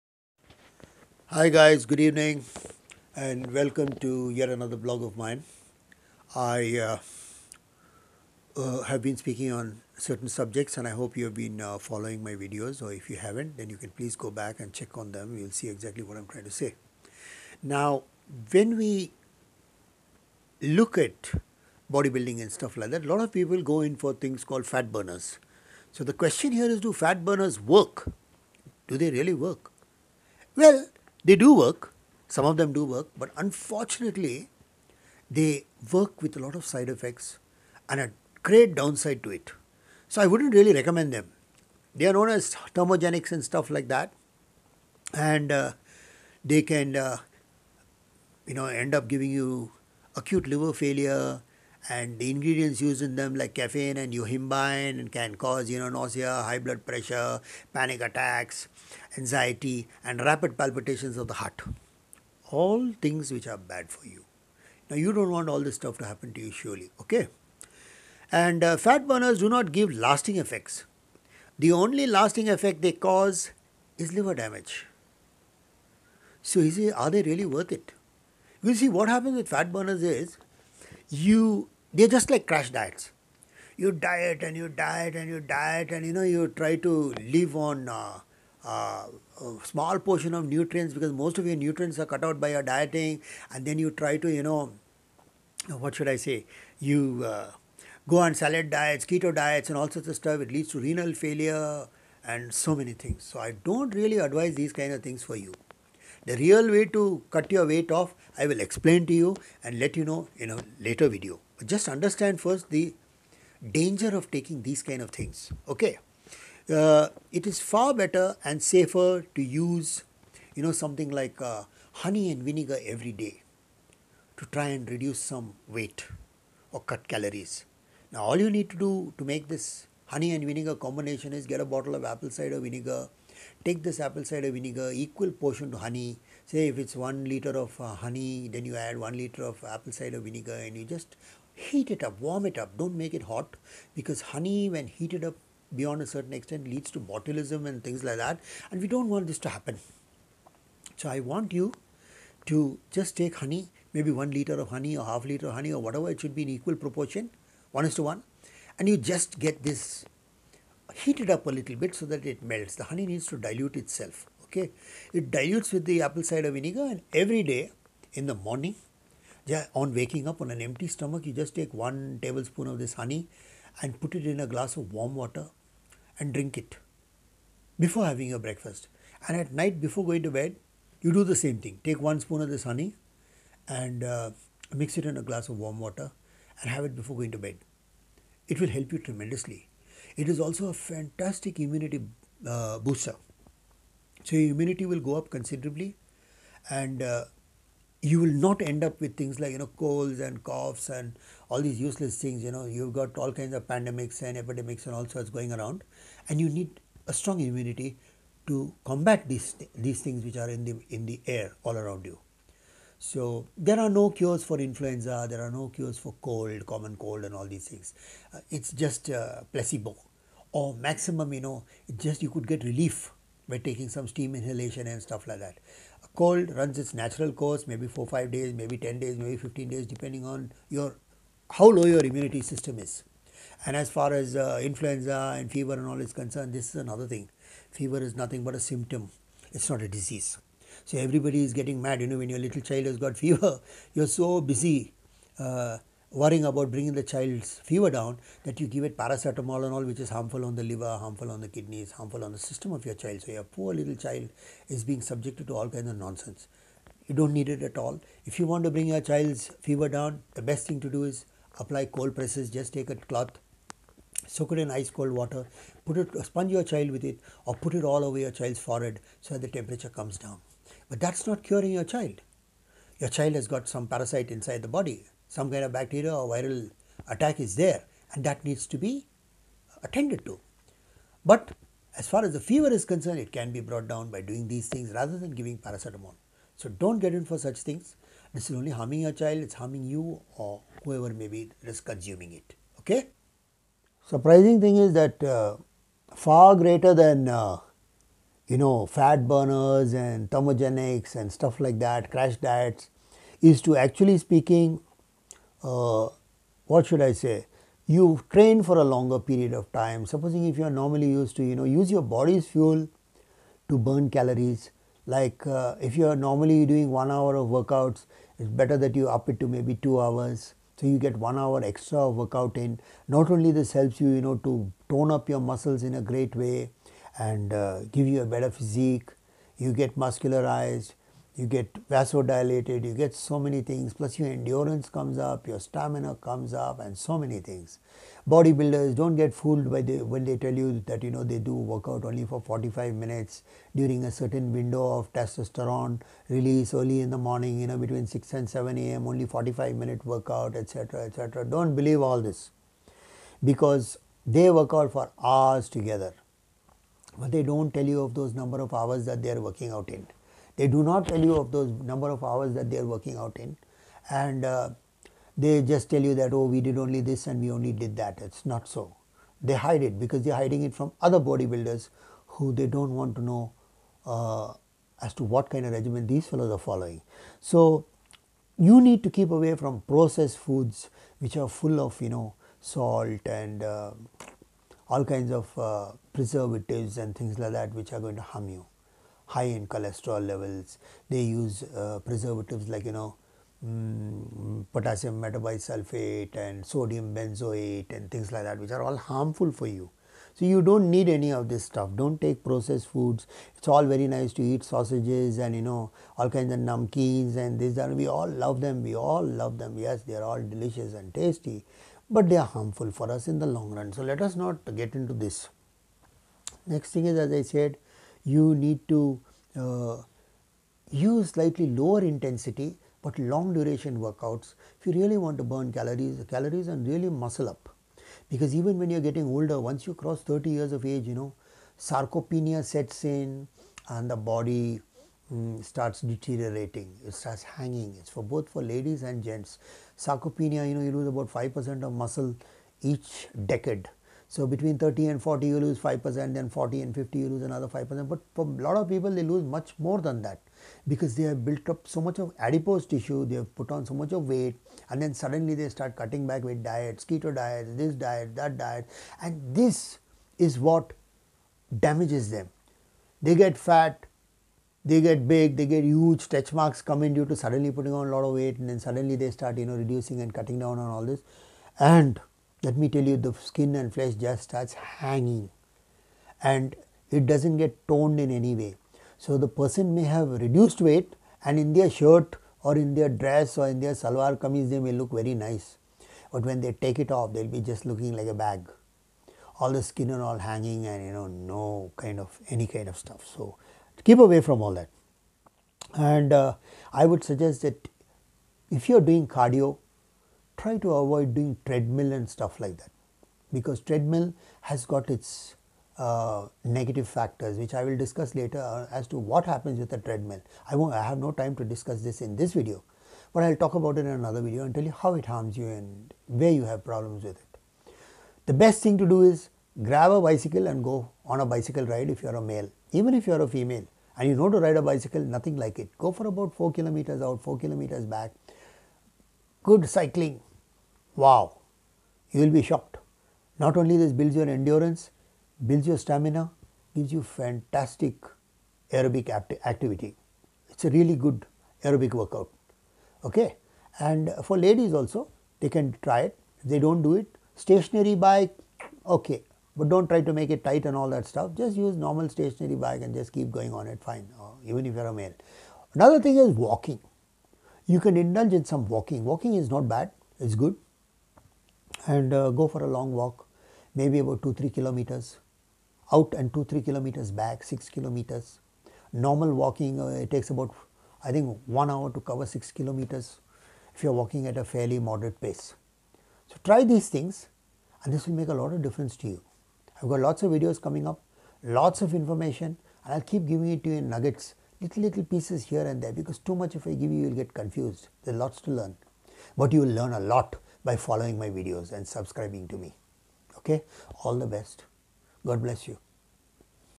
HOW FAT BURNERS HARM AND CRASH YOU.
Before you use fat burners and crash diets understand them by watching this video.